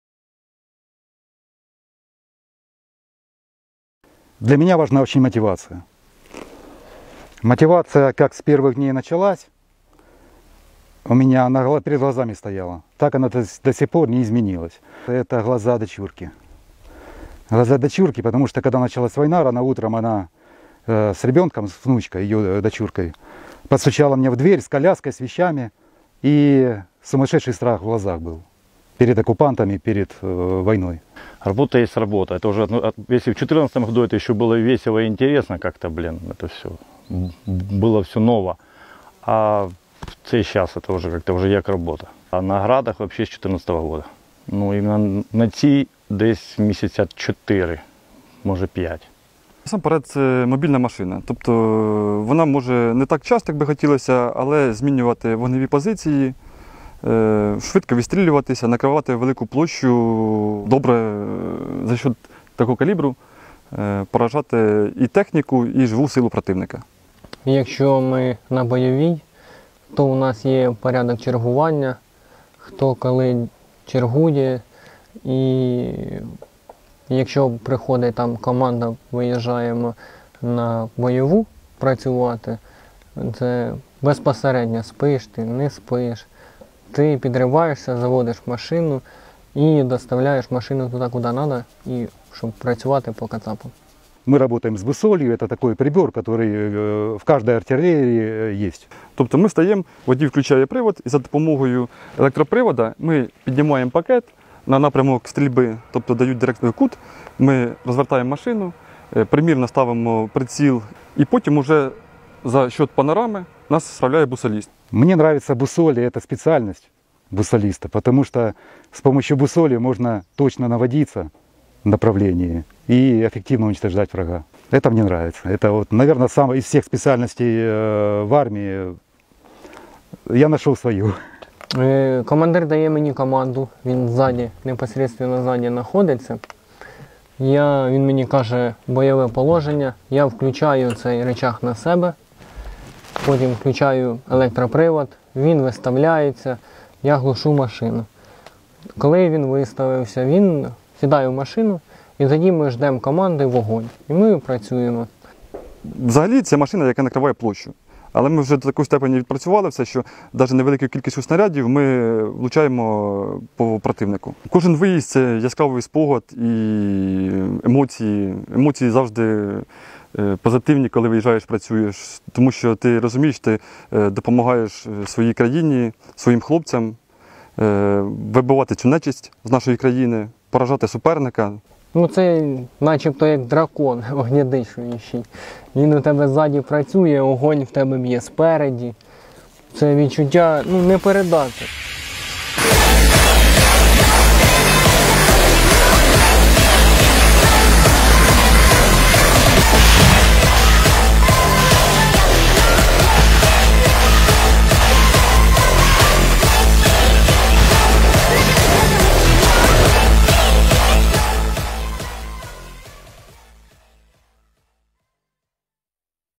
Бійці 60-ої ОМБр діляться тонкощами ефективного вогневого ураження позицій ворога
Бійці 60-ої окремої механізованої Інгулецької бригади, що працюють на РСЗВ “Град”, діляться тонкощами ефективного вогневого ураження позицій ворога та роботи на установці в цілому.